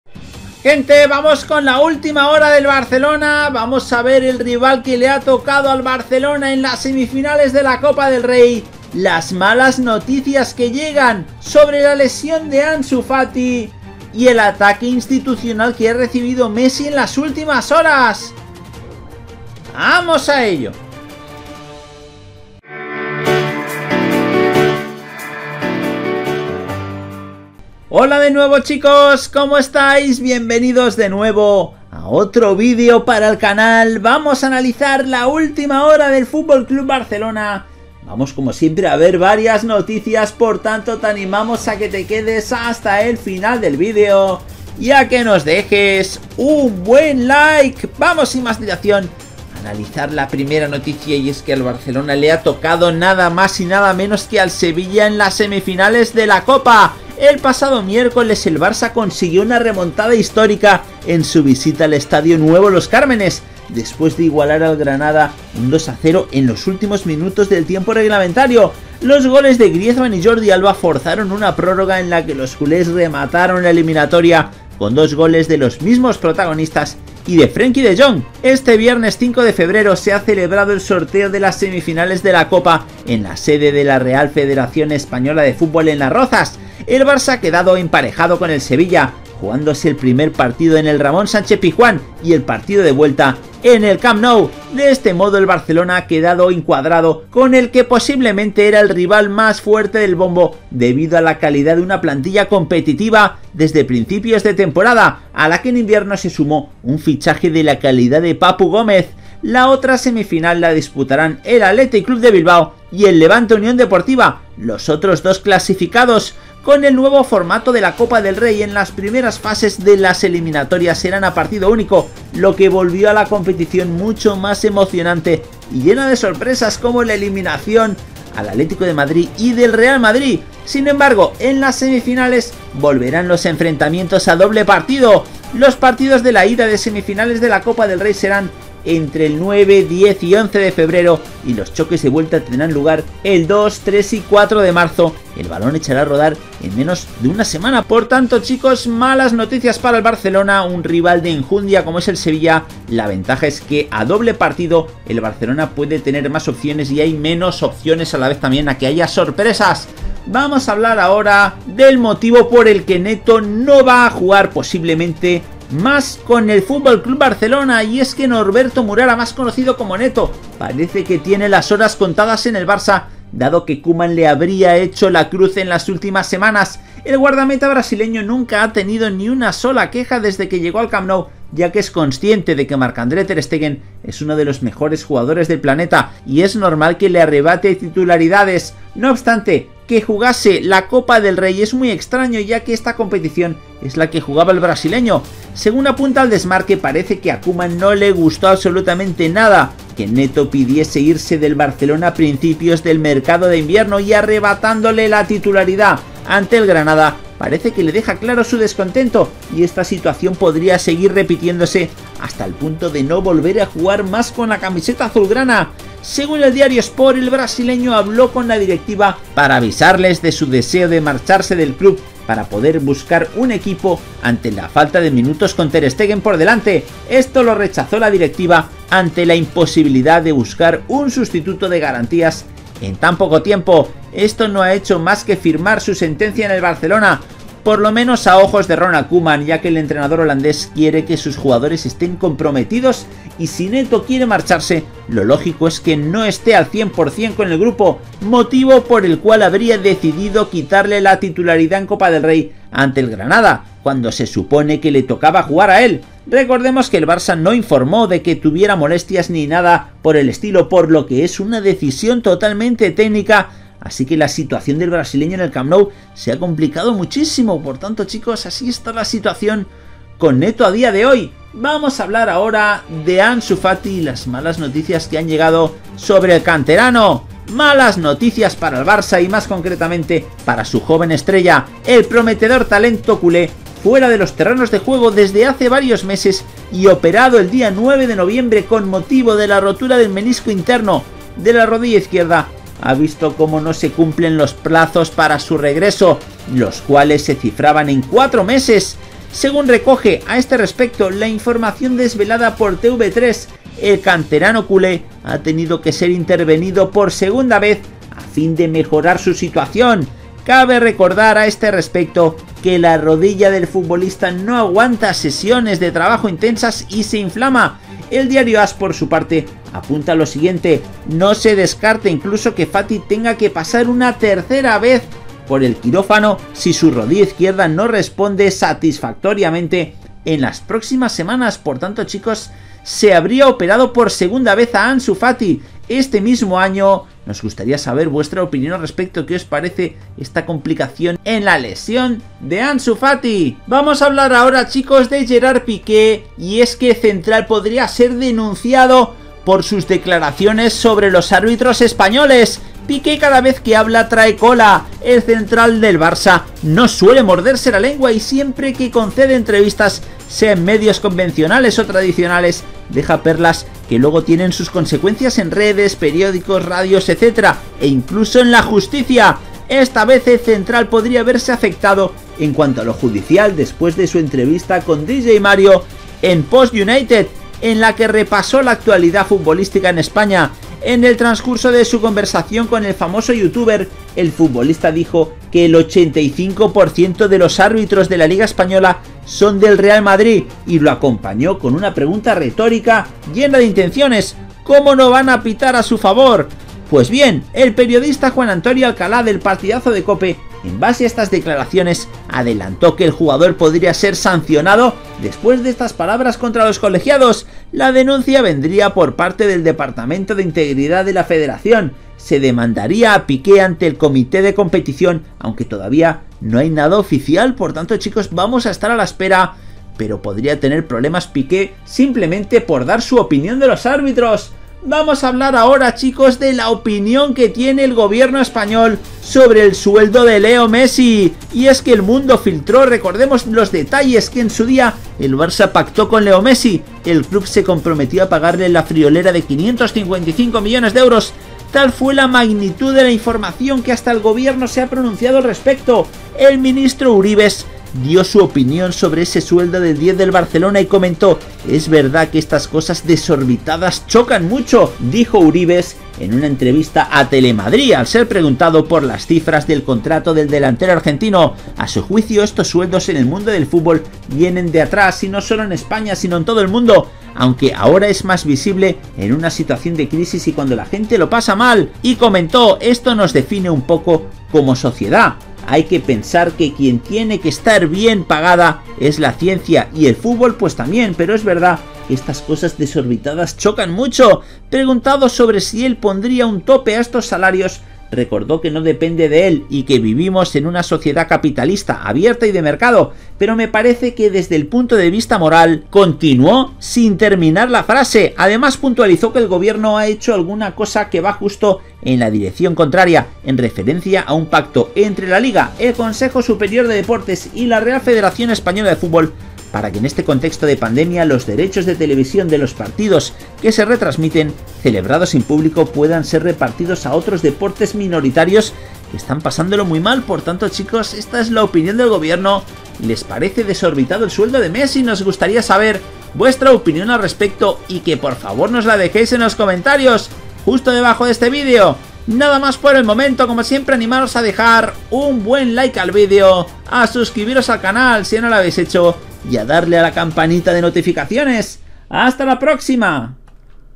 🔴MESSI RECIBE PALO BRUTAL INSTITUCIONAL⚽OFICIAL:MALAS NOTICIAS para el BARÇA 🔥DRAMA CON ANSU FATI 🔥
Fuentes:
besoccer
fcbarcelonanoticias
ficahjes com
fichajes net